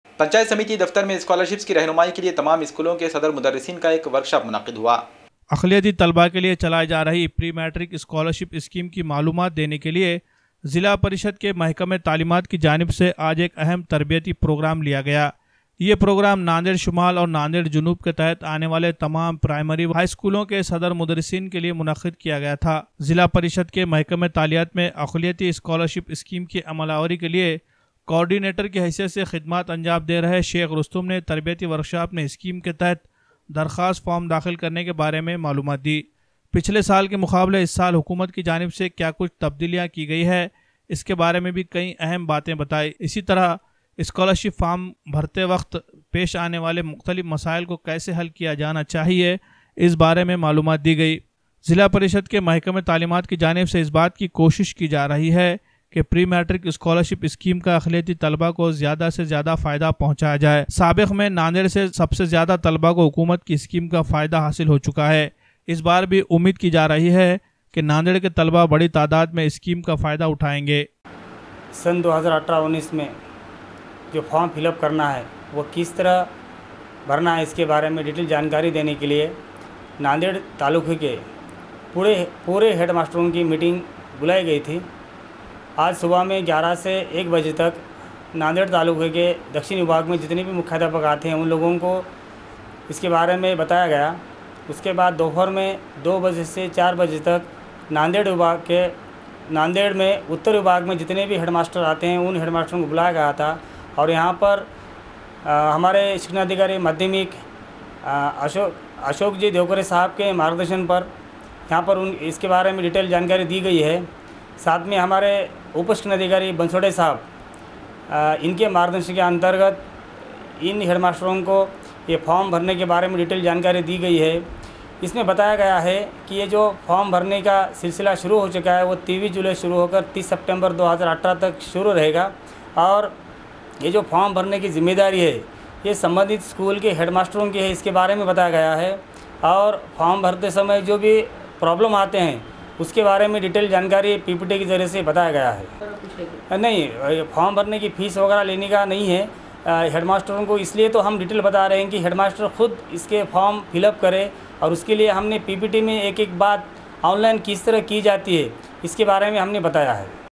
प्रेमट्रिक स्कॉलर शिप स्कीम की जानकारी देने के लिए कार्यषाला का आयोजन
जिला परिषद् शिक्षण विभाग की औरसे